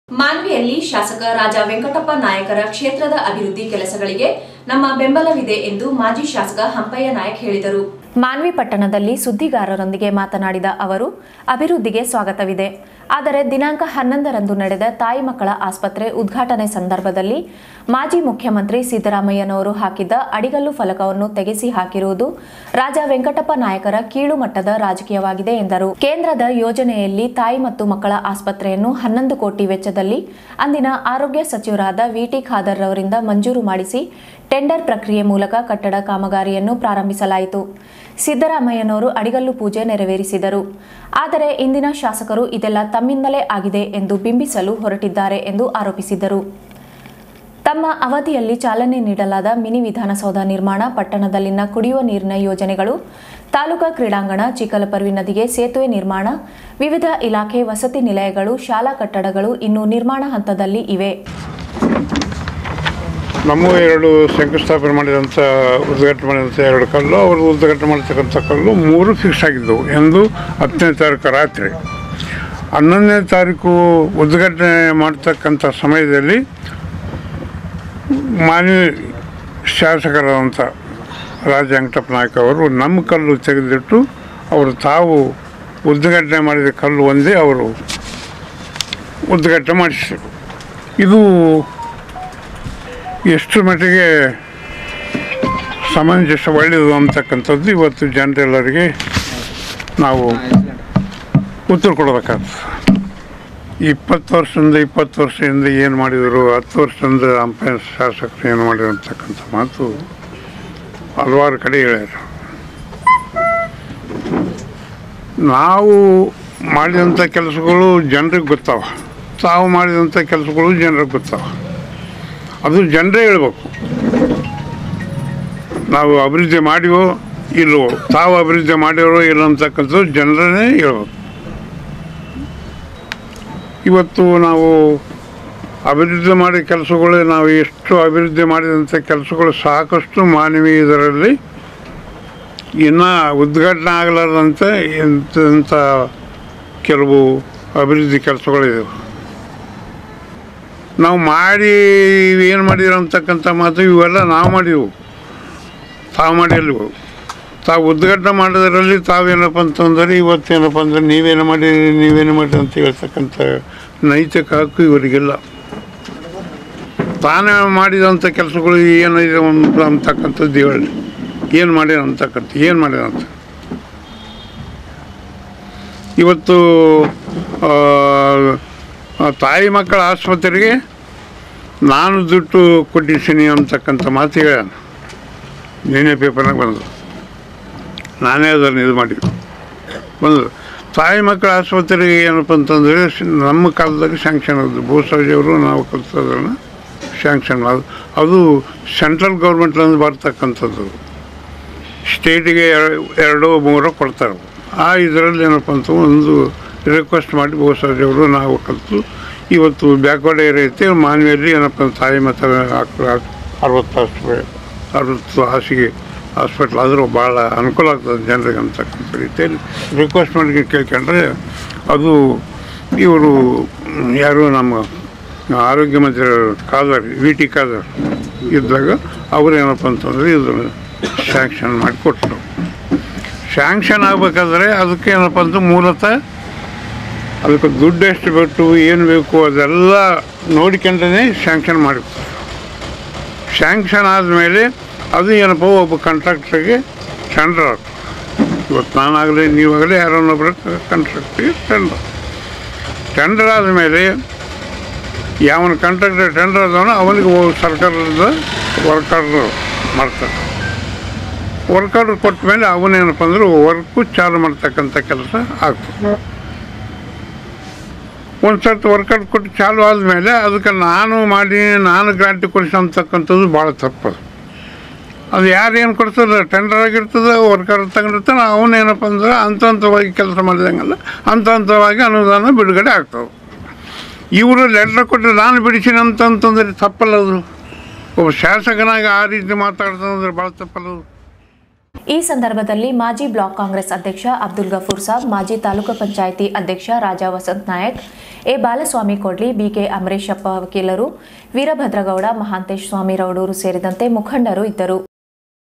ಮಾನ್ವಿ: ಶಾಸಕ ರಾಜಾವೆಂಕಟಪ್ಪ ನಾಯಕರ ಕ್ಷೇತ್ರದ ಅಭಿವೃದ್ಧಿ ಕೆಲಸಗಳಿಗೆ ನಮ್ಮ ಬೆಂಬಲವಿದೆ ಮಾಜಿ ಶಾಸಕ ಹಂಪಯ್ಯ ನಾಯಕ
MANVI NEWSನಾಮಫಲಕ ತೆರವಿಗೆ ಮಾಜಿ ಶಾಸಕ ಆಕ್ರೋಶ, ಅಭಿವೃದ್ಧಿಗೆ ನಮ್ಮ ಬೆಂಬಲವಿದೆ : ಮಾಜಿ ಶಾಸಕ ಹಂಪಯ್ಯ ನಾಯಕ